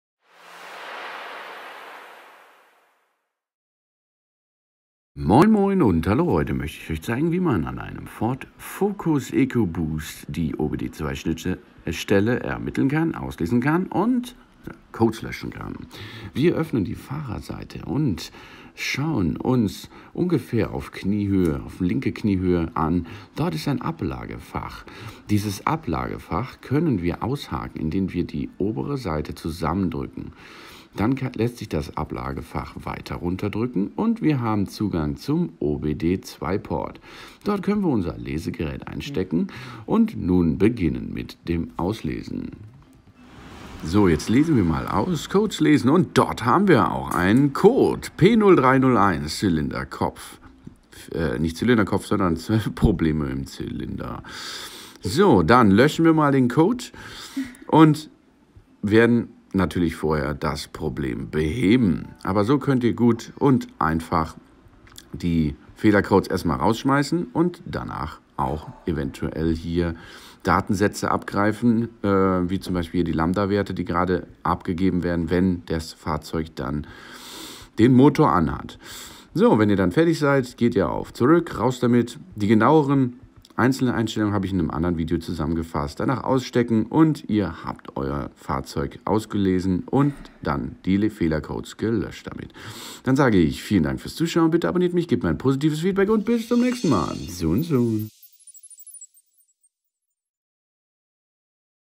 PKW OBD2 Fehlercodes auslesen KFZ Fehlerspeicher auslesen und löschen Ford Focus Eco Boost Anleitung
Zubehör:

OBD2 Scanner

Der Ford Focus ist eine Pkw-Modellreihe des Automobilherstellers Ford in der Kompaktklasse. Seine Markteinführung fand in Europa im Herbst 1998 statt, in Nord- und Südamerika im Frühjahr 2000 und in Australien im September 2002.

In den meisten Ländern war er der Nachfolger des Ford Escort, in Asien und Australien ersetzte er den Ford Laser. Im April 2011 erfolgte sowohl in Europa, den USA als auch in Asien die Einführung der derzeitigen dritten Generation des Kompaktwagens. Im Jahr 2014 wurde eine Modellpflege vorgenommen.

In den Jahren 2012 bis 2014 erreichte der Ford Focus mit jeweils mehr als einer Million verkauften Exemplaren erneut den ersten Rang in den weltweiten Verkaufsstatistiken.

Quelle: Wikipedia

pduesp